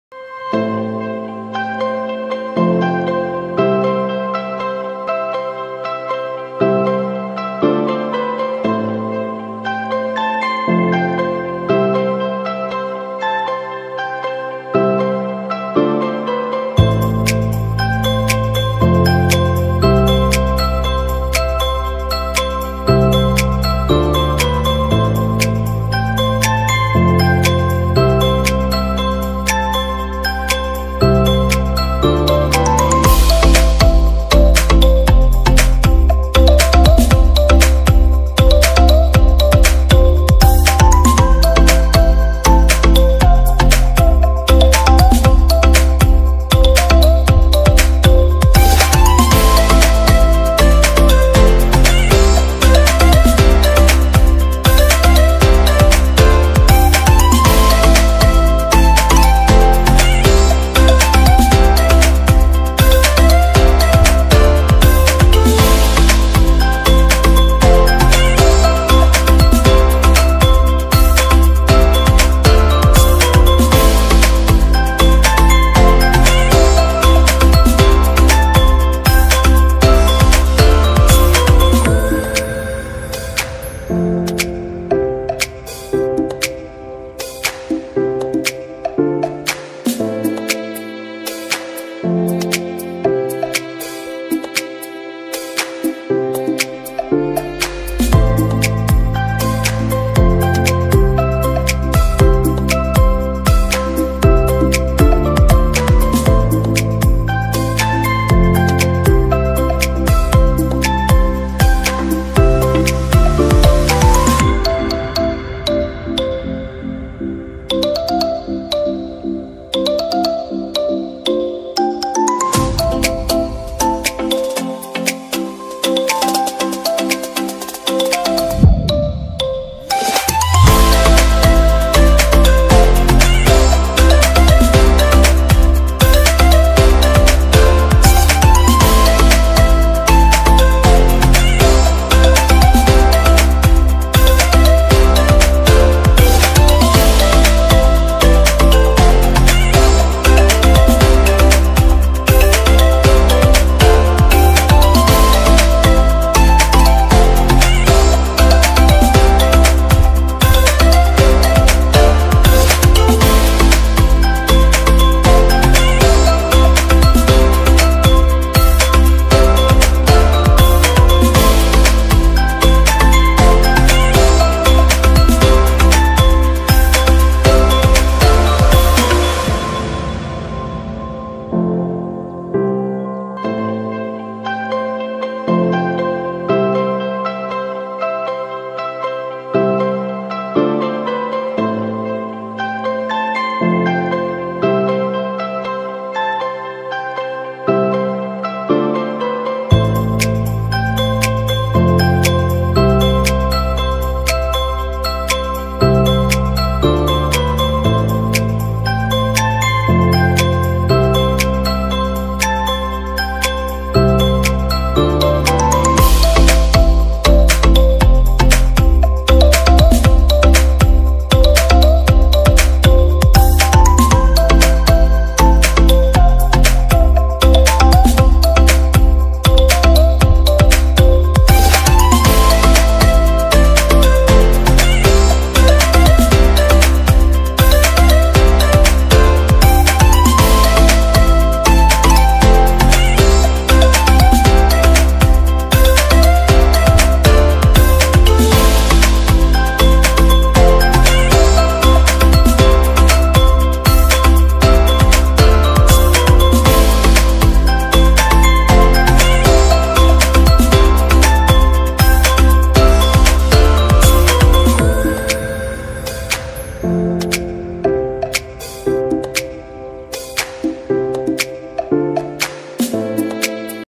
Small bee's nest at our orange tree!They are living together with the insect's family!
animal at my tree.

In this video we showing you about an insect life because I love the insect.